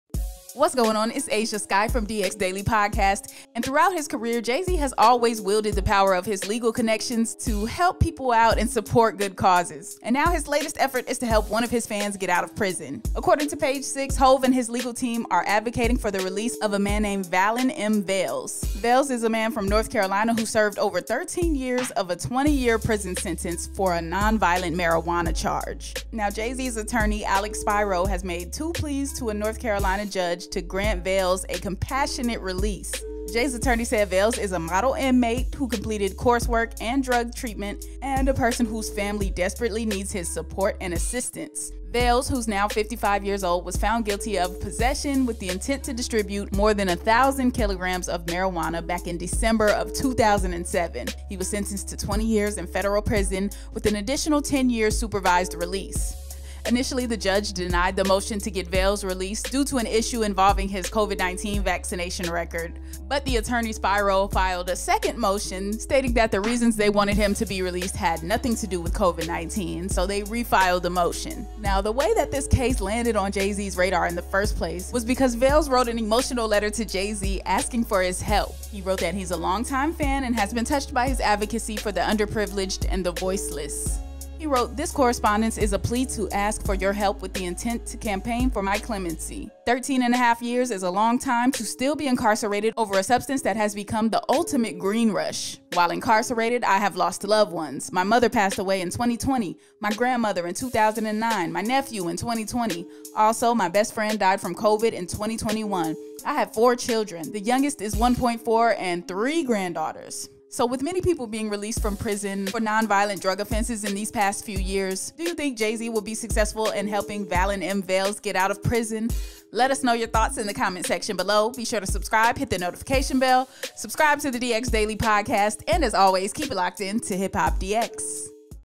JAY-Z Fighting To Free Incarcerated Fan Serving 20 Years For Non Violent Offense
JAY-Z has been wielding the power of his legal connections for various good causes in recent years, and his latest effort is to help free one of his fans from prison.

Script: Andy Bustard
Voiceover: Ashia Skye